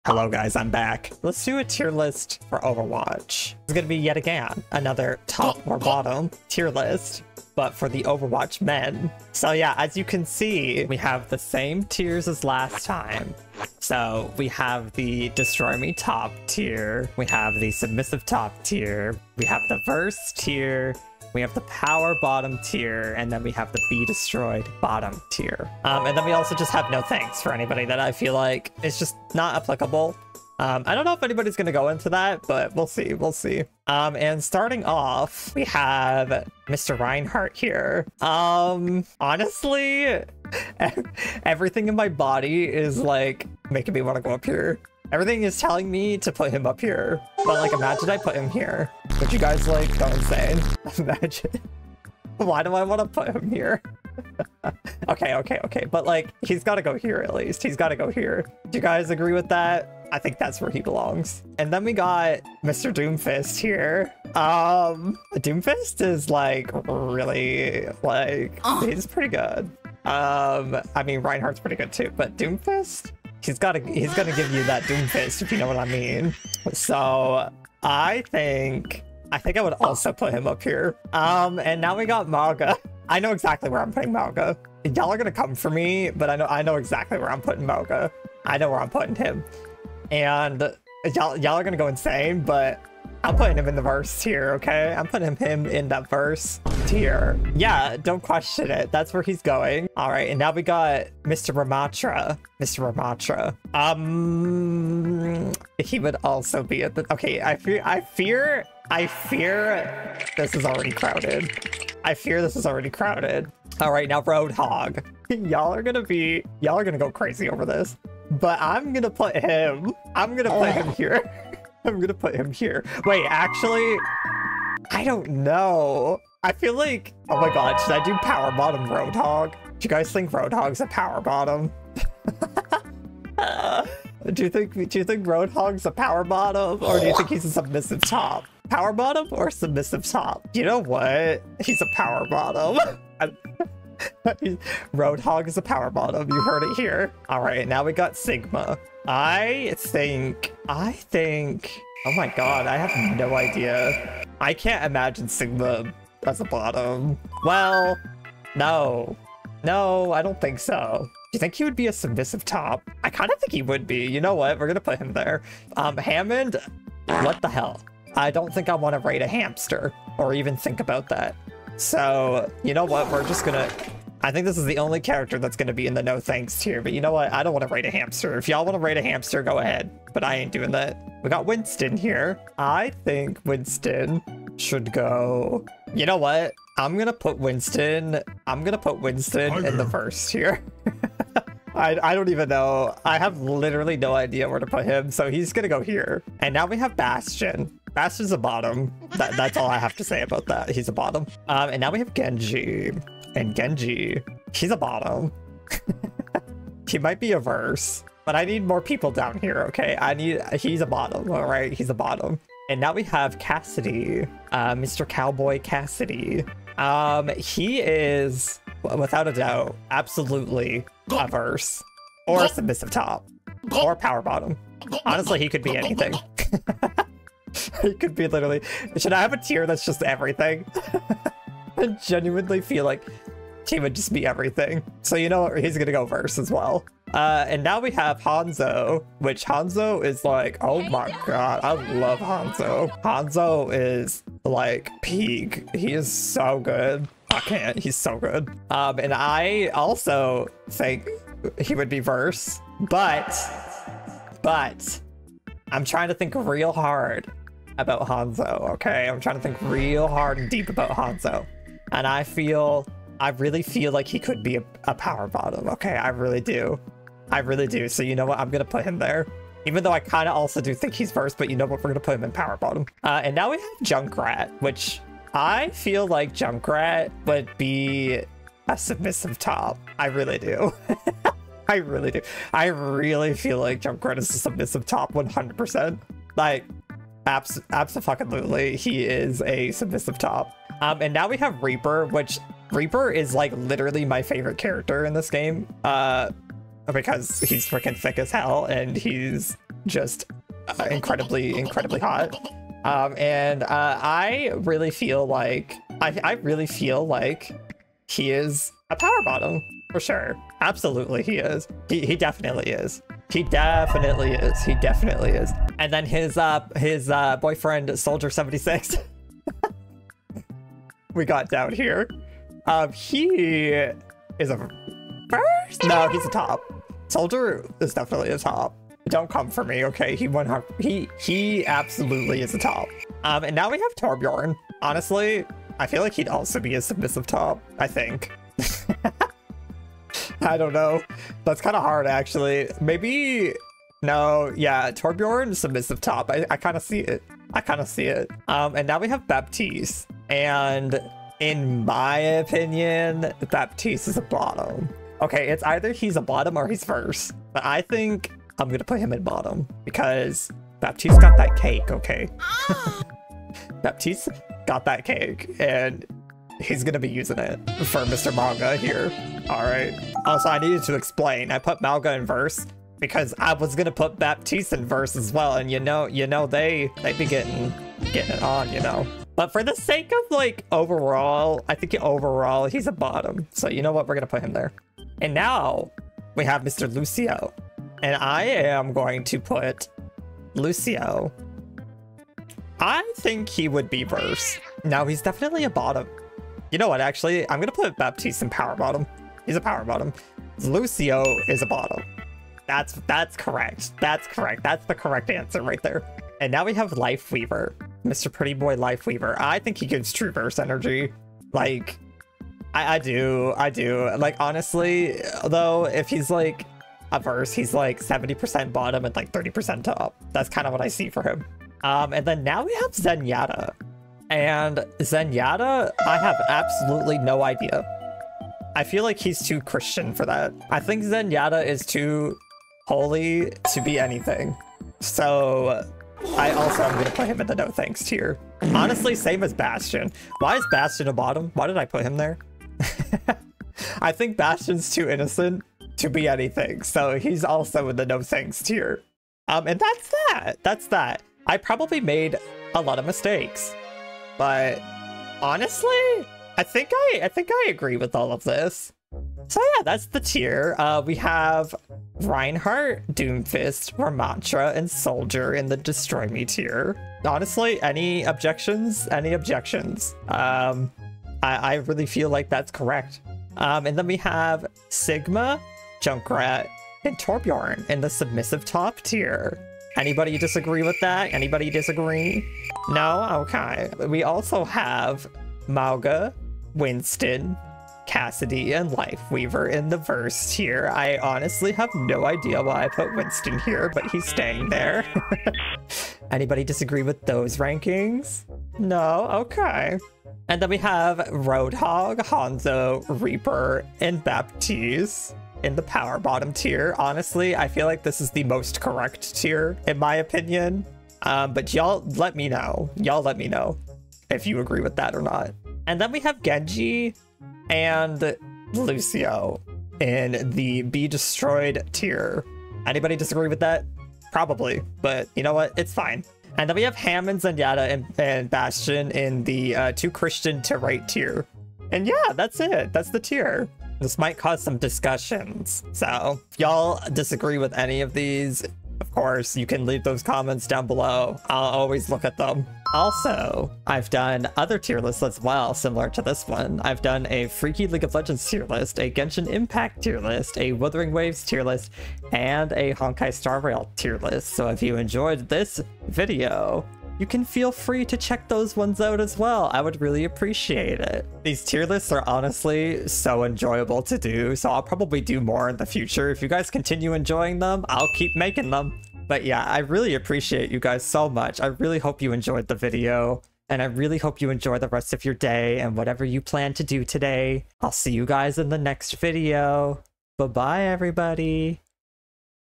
A FREAKY Overwatch 2 tier list (male characters)
We are here again delivering ANOTHER freaky tier list! This time it's Overwatch 2!
I honestly ended up disagreeing with the placements I did while editing this video LOL

If you want to see my other tier lists like this one!!

If you haven't already I would love if you went and followed my twitch. :)